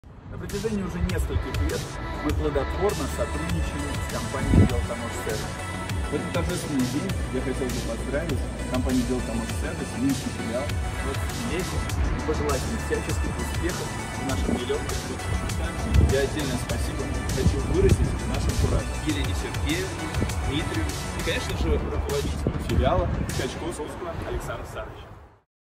BTS 15.10.2020.
Продолжаем креативить с видеопоздравлением от China Logistic. Погода внесла свои коррективы, поэтому выбрали Дворцовую площадь 🎥🎥🎥